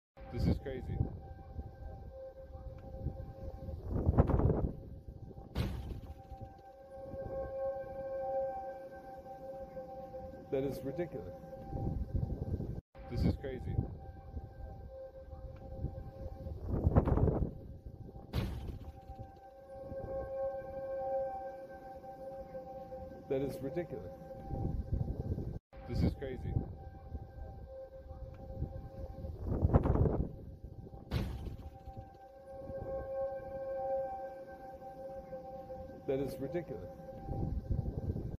Tornado Sirens Wail, Amazon Driver Completes Delivery
Amazon driver seen delivering a package in Omaha, Nebraska during a tornado-warned storm.